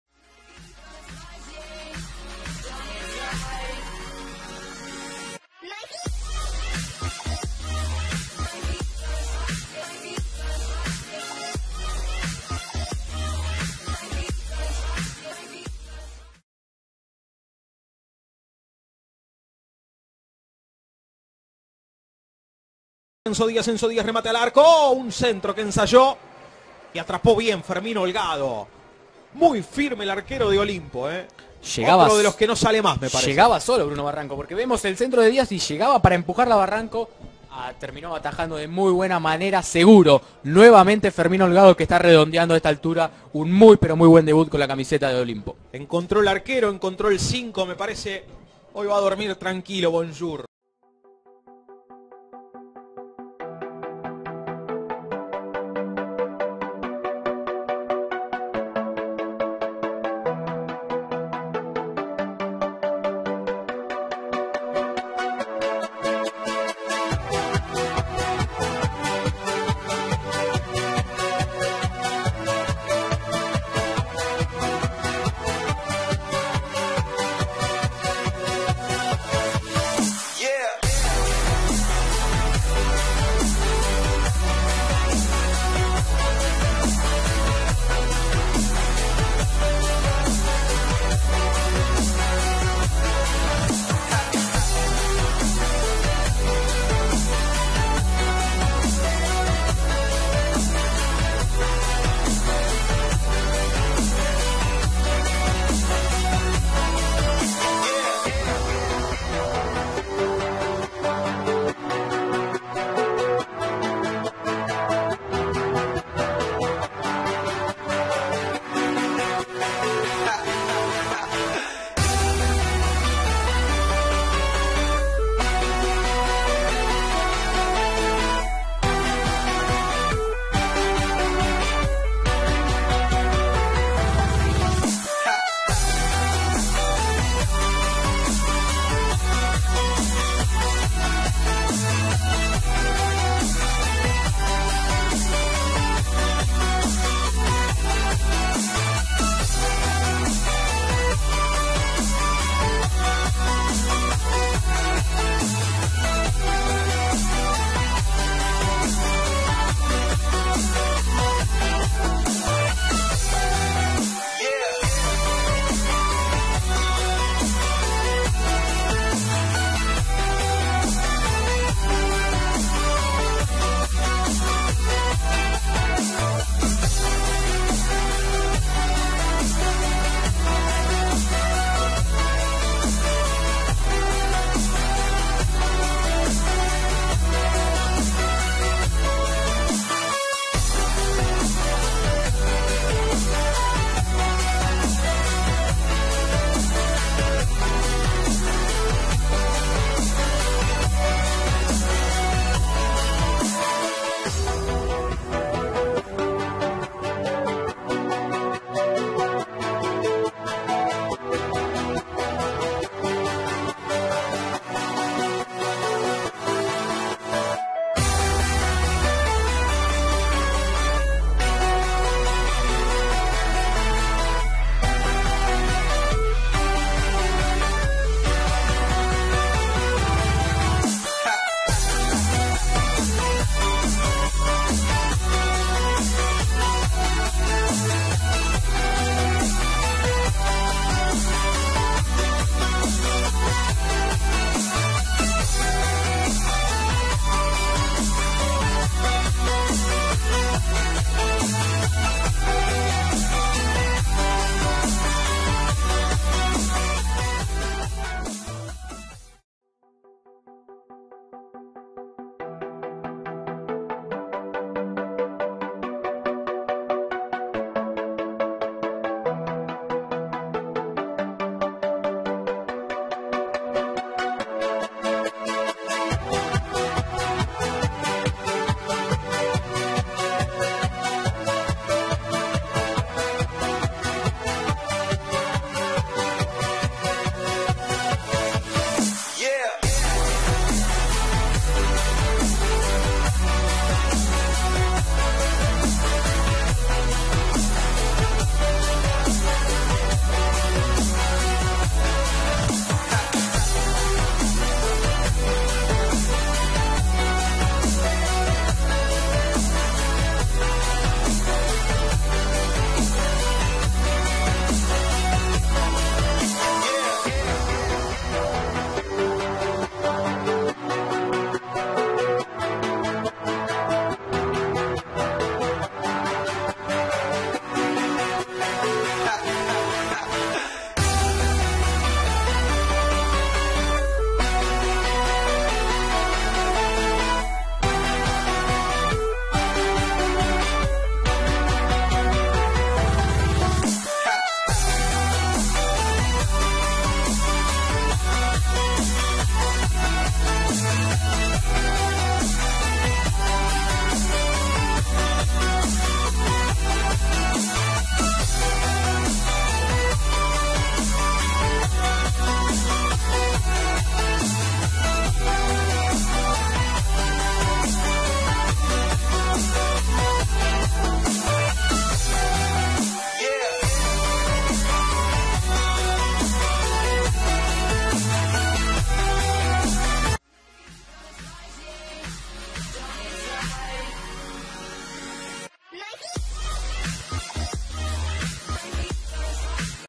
Fermin Holgado vs Ferro Carril Oeste - Debut Olimpo (Arg) - 17/09/2018
FERMIN HOLGADO (OLIMPO  VS FERRO CARRIL OESTE)
16/09/2018   FECHA n°3 (B Nacional)

Fermin Holgado, el arquero que llego a Olimpo para quedarse
Hoy analizaremos el juego del arquero debutante en Olimpo. Su nombre es Fermin Holgado, tiene 24 años, nacido en Trenque Lauquen el 17/05/1994 . Arquero de gran porte y  buen juego con los pies, especialmente su gran pegada, que se ve reflejada en la cantidad de pases largos con precisión que efectivizo en su debut enfrentando a Ferro.
Podemos ver como el equipo de Bonjour tiene una preparación a la hora del juego aéreo, y aprovechan la pegada del arquero para jugar balones aéreos cerca del área. En los saques de arco o “goal kicks”, el arquero del equipo bahiense dirigía los pases largos frecuentemente hacia el costado derecho de la cancha, pelotazos abiertos que por lo general buscaban a Graciani (uno de los mejores cabeceadores del equipo), al parecer es una de las salidas ensayadas del equipo de Bonjour, que le ha dado buenos resultados, creando al menos tres situaciones claras de gol a partir de esta salida. Esto se puede ver en el minuto 26 del ST , cuando Fermin , antes de sacar el goal kick ,traslada la pelota hacia el lado derecho del área chica, para realizar esta salida ensayada ( Lo hizo en todos los goal kicks pero para hacer mas resumido el video, cortamos esa parte, y solo la dejamos de ejemplo en esa ocasión).
Se trata de una forma de iniciar el ataque, que ponen en practica como segunda opción, cuando no pueden salir jugando desde el fondo, ya sea por la presión del equipo rival en la salida, o simplemente porque les funciona mejor el saque largo al ser superiores en ese ámbito del juego.
Es destacable ver como un entrenador analiza el partido previamente de una manera, pero es flexible y durante el partido se da cuenta que amerita jugar de otra forma, en este caso se dio cuenta que la salida por abajo no era tan efectiva como si lo era el saque largo bien dirigido, y aprovechando la calidad de holgado maniobro en pleno partido, esto es un punto a favor para Bonjour.
Ademas de la calidad de su pegada, podemos destacar su habilidad a la hora de cortar centros, por ejemplo en el minuto 30 del ST, donde corta magníficamente un centro que podía ser peligroso, y sale sin titubear , a pesar de atajar por primera vez en el arco de Olimpo, ya demuestra tranquilidad y seguridad, y sobre todo estar a la altura de semejante responsabilidad.
A los 39 del ST , Ferro tuvo una clara para descontar, pero Holgado respondio bien, atajando un mano a mano , en principio fingiendo descubrir el primer palo, pero luego achicando muy bien , dejando al delantero prácticamente sin opciones claras para colocar la pelota, y atajando el disparo con los pies.
A modo de conclusión, podemos decir que Holgado tuvo un buen debut, que es un futbolista que trajo tranquilidad al arco del aurinegro, con buena pegada, seguro a la hora de salir en los centros, hábil en el 1 a 1 , rápido a la hora de cortar pelotazos largos a la espalda de los defensores (mas alla de medir 1,96) y con mucha personalidad. El dt del equipo bahiense estará conforme con el partido del arquero de Trenque Lauquen ,tal y como lo manifestó en la conferencia de prensa.
Si quieren saber mas sobre el arquero y su trayectoria, dejamos el link abajo: